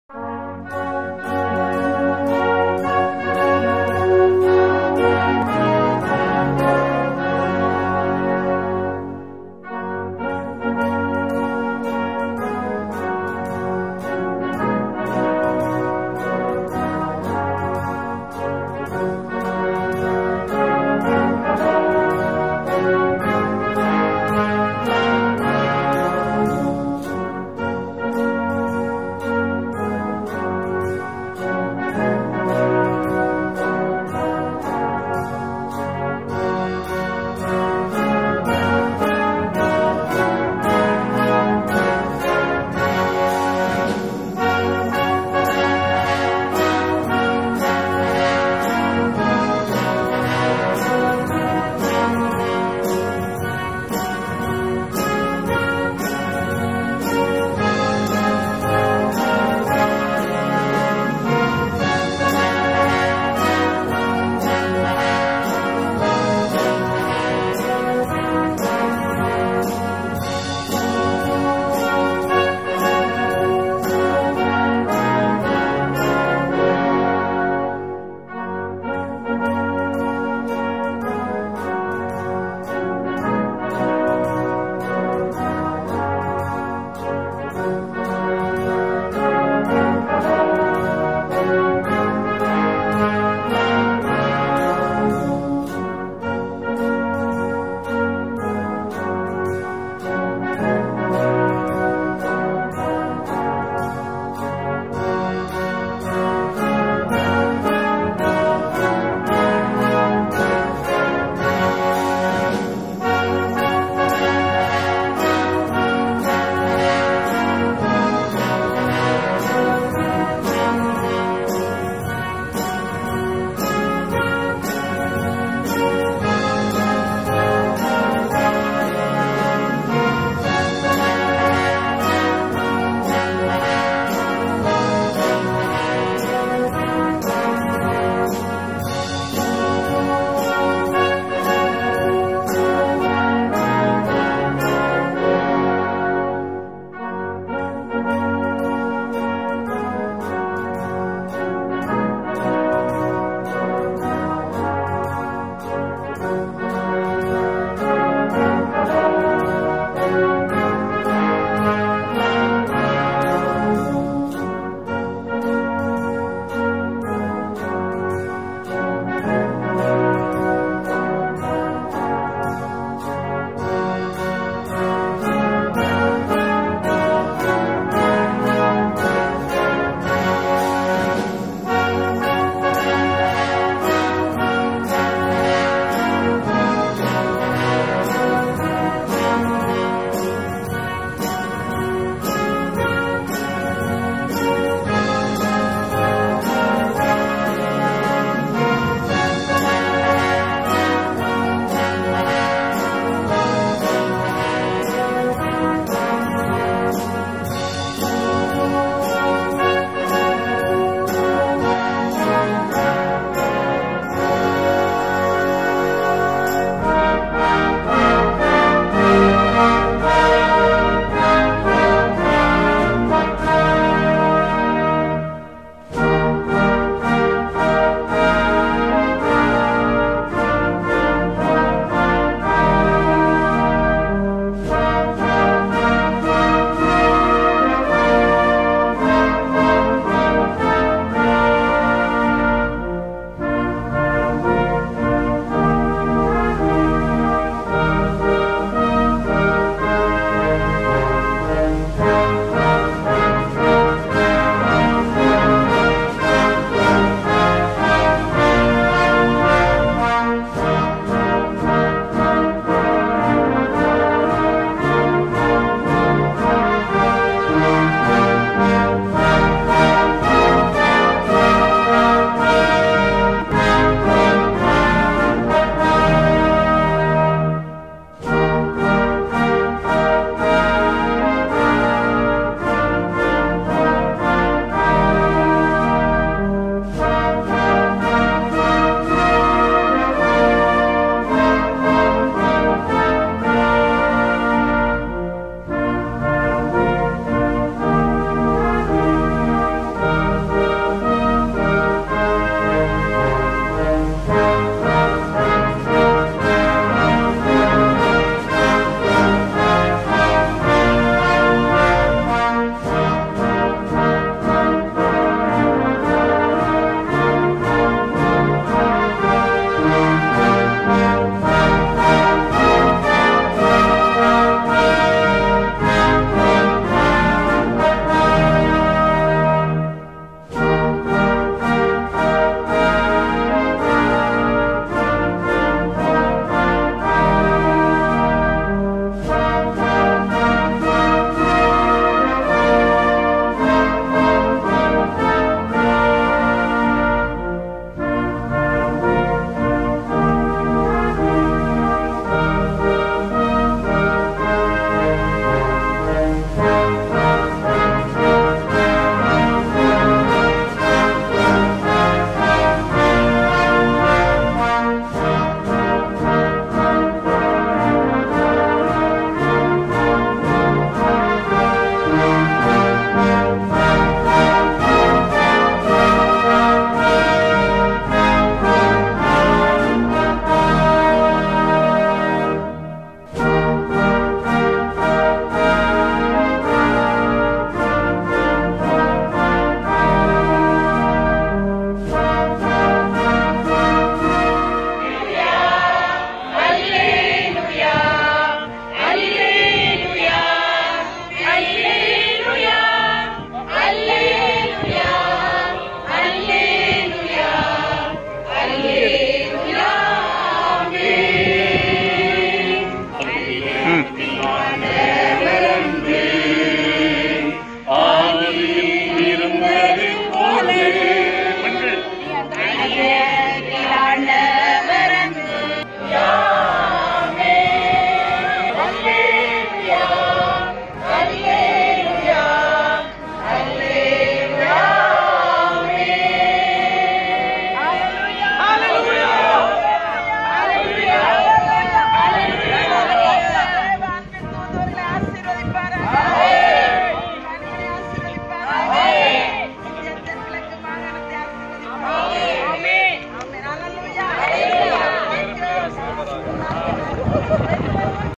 Messengers of God's Love | Session 1995-1997 | 25 Year's Long Service Order |The Salvation Army ISET
India South - Eastern Territory

Messengers of God's Love Session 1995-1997
25 Year's Long Service Order

Conducted by
Commissioner John Kumar Dasari, Territorial Commander
Commissioner Mani Kumari Dasari, TPWM

Lieut-Colonel  Lalbulliana Tlau, Chief Secretary
 Lieut-Colonel Lalnunhlui Khawlhring, TSWM

Supported by
Personnel Department - ISET